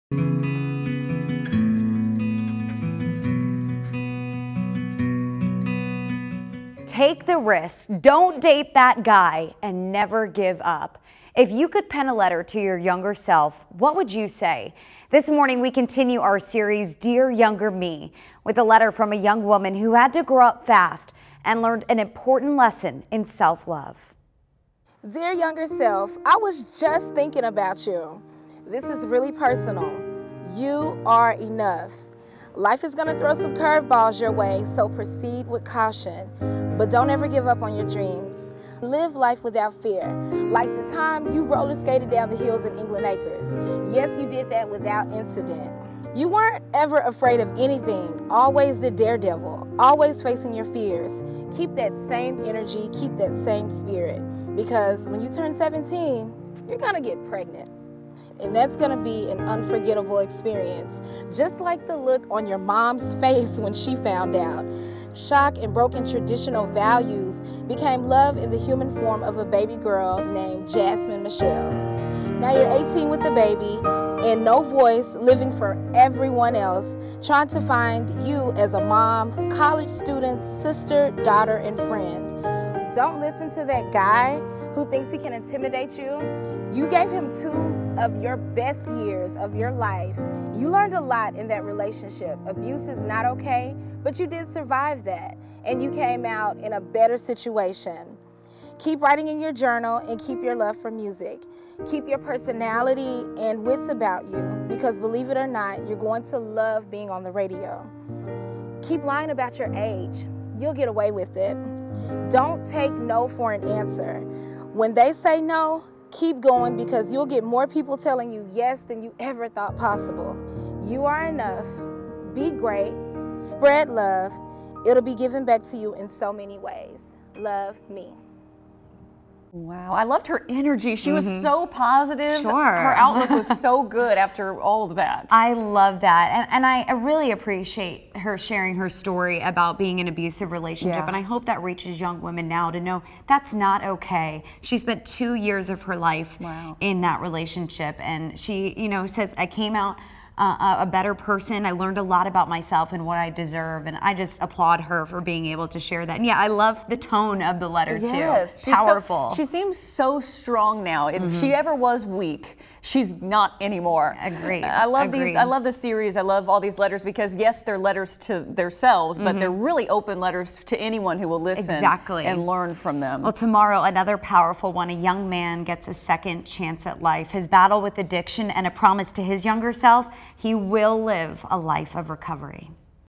Dear Younger Me: Idris
Take the risk.
 Don't date that guy.
 If you could pen a letter to your younger self, what would you say?
 This morning  we continue our series Dear Younger Me with a letter from a young woman who had to grow up fast and learned an important lesson in self love.